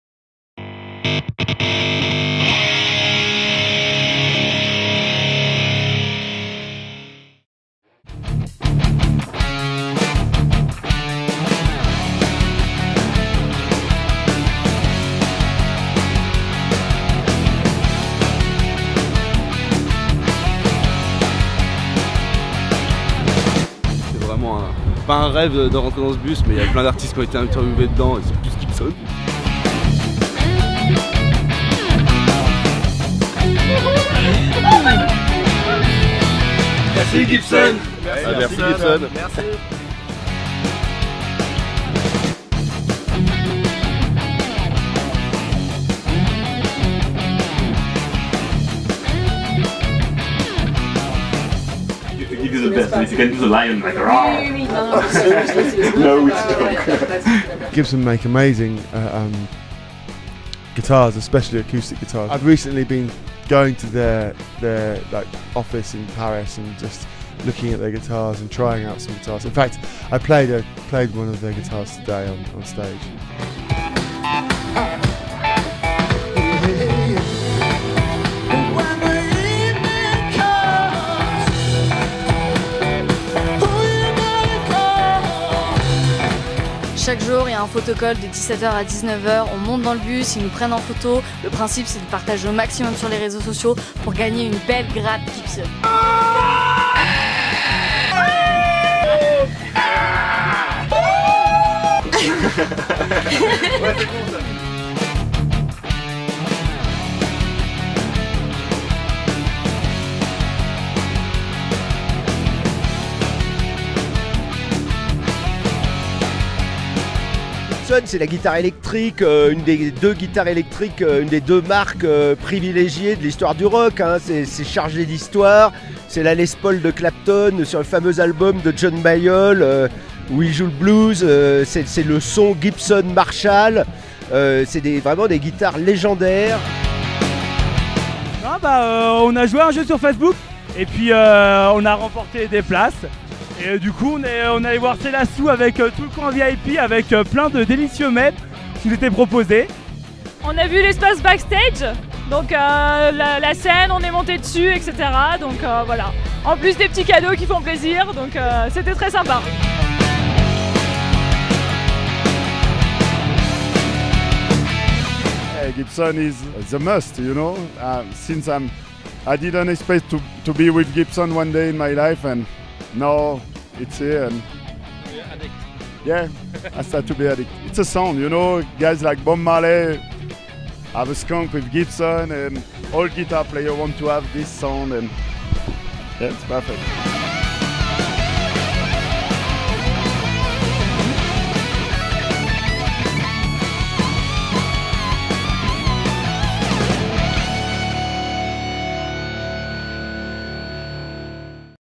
Au coeur du festival Rock The Pistes avec Gibson !
Du 14 au 22 mars 2015, Gibson était partenaire du festival Rock the Pistes à Avoriaz ! Certains d'entre vous ont même eu la chance de faire route dans le bus Gibson avec nous, remporter des pack VIP et gagner trois belles Les Paul ! Retour en image sur cette semaine de folie aux Les Portes du Soleil​ avec Charlie Winston, The Do, Brigitte, Selah Sue, Dub Incorporation, mais aussi Philippe Manoeuvre, Miss America et  tant d'autres !